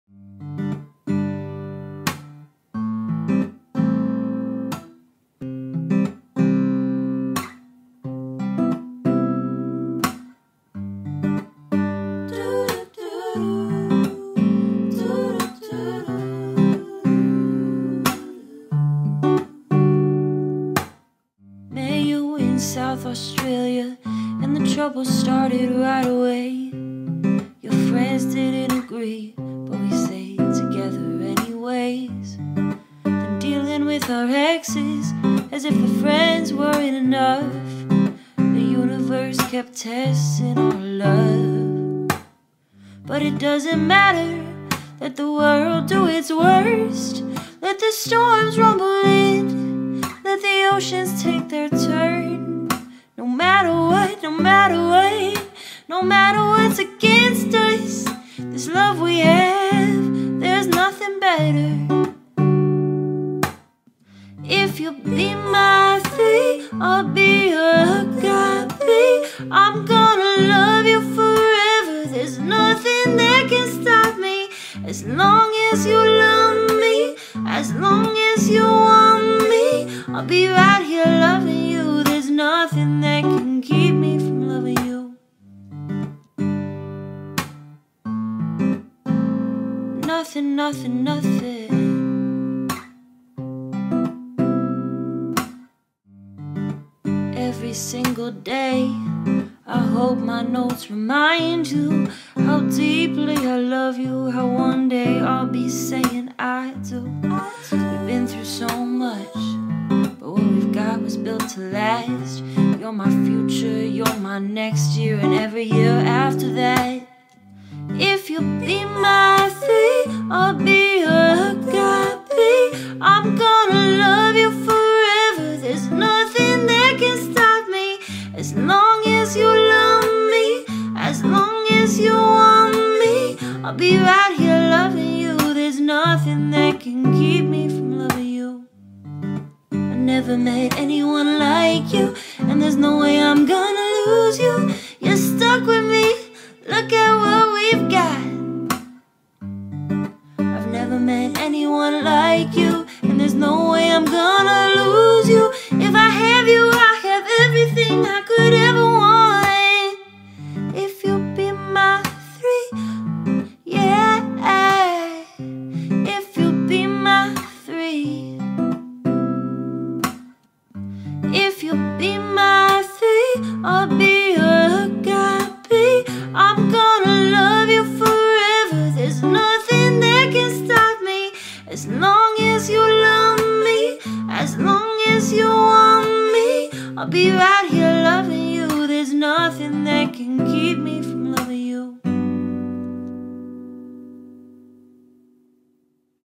K&G our love song
A song about us and all the trouble we went through to get here ..